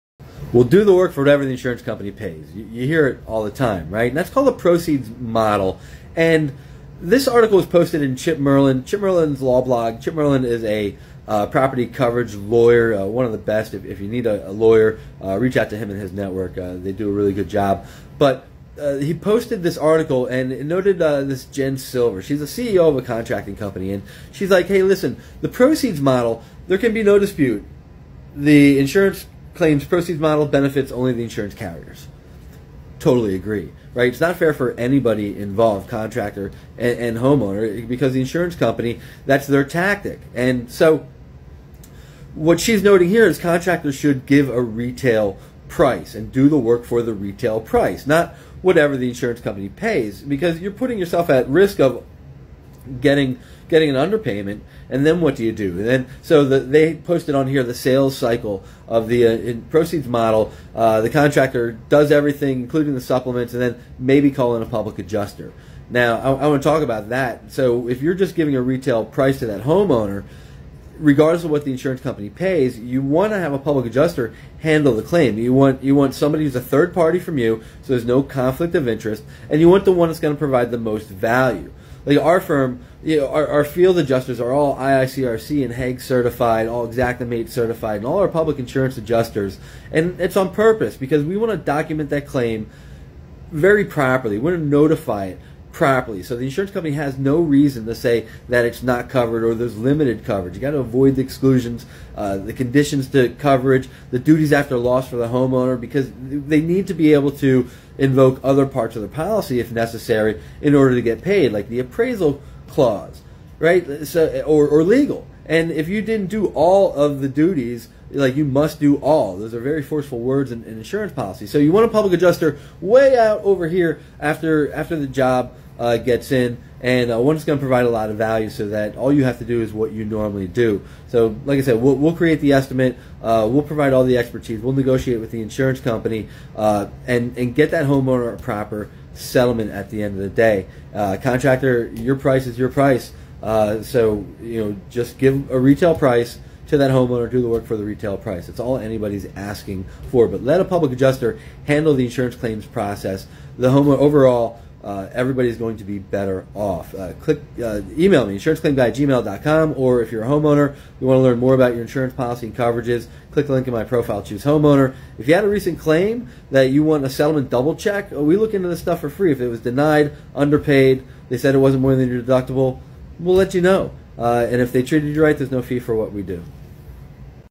Contractors Proceeds Contracts Only Benefit The Insurance Company
Allowing the insurance company to control the claim is detrimental to home owners and their contractors.